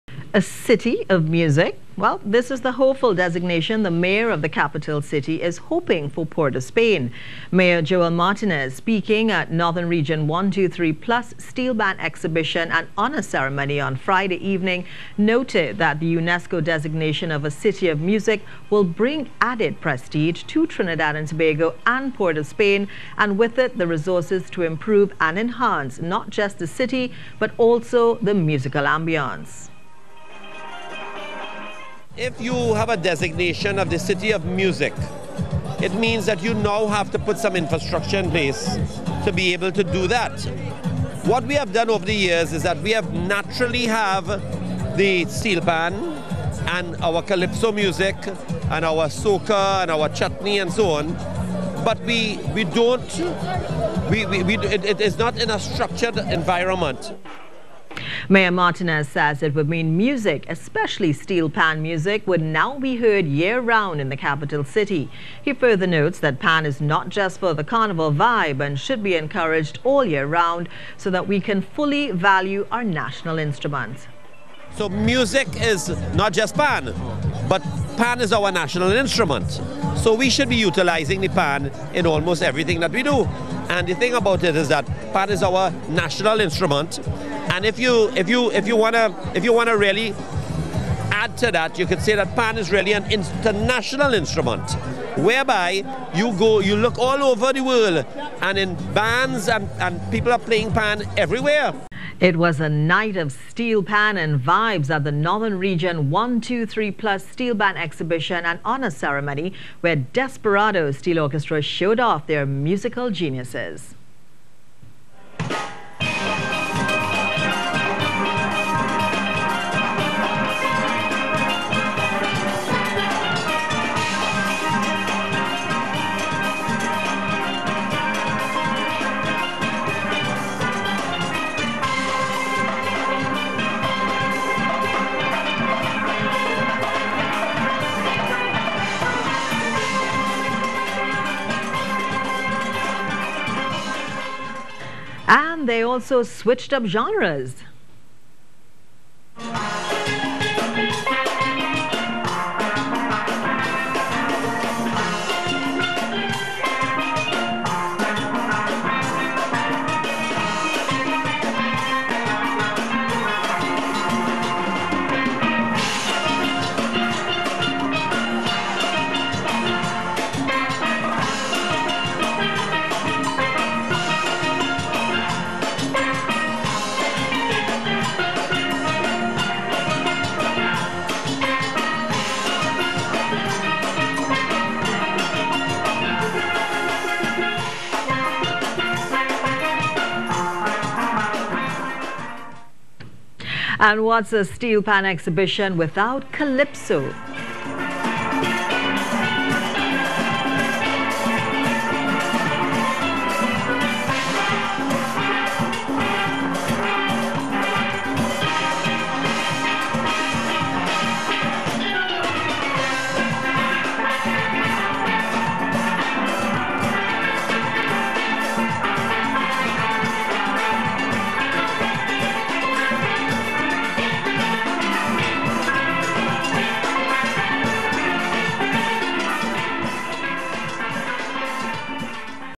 Building A City Of Music
A city of music is the hopeful designation the Mayor of the capital city is hoping for Port of Spain.

Speaking at the Northern Region 1,2,3 Plus Steelband Exhibition and Honour Ceremony on Friday evening, Mayor Joel Martinez noted that the UNESCO designation of a city of music will bring added prestige to Trinidad and Tobago and Port of Spain and, with it, the resources to improve and enhance not just the city but also the musical ambiance.

Mayor Martinez says it would mean music, especially steel pan music, would now be heard year round in the capital city.

He further notes that steelpan is not just for the Carnival vibe, and should be encouraged all year round so that we can fully values our national instrument.